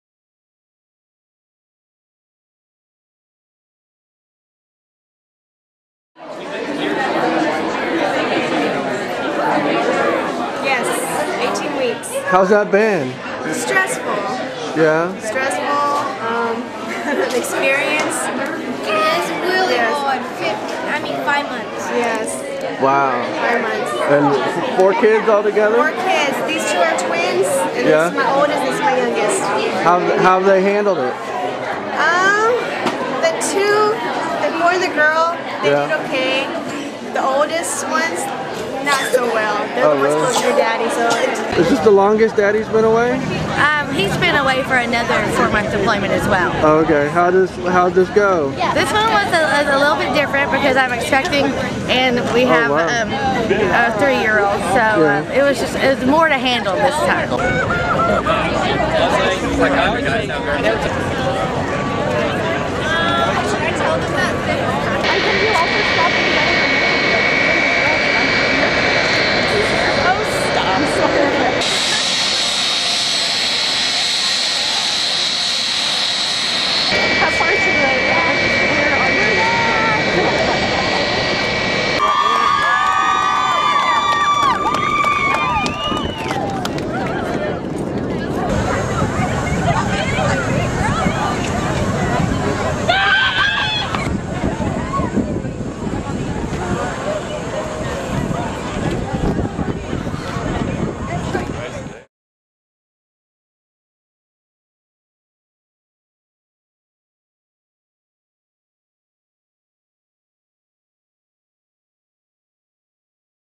Troops Return
Cheers, hugs and kisses welcomed the 16th Airlift Squadron back to Charleston.